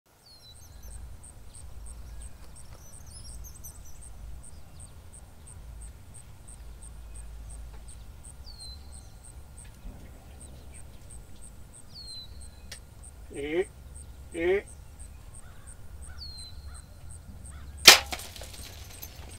Strategy Works! Big Buck comes in to check a mock scrape then...
A mock scrape can be a great tool to pattern and attract bucks and does into range!  If you have never used one check out the linked video to see how to make one,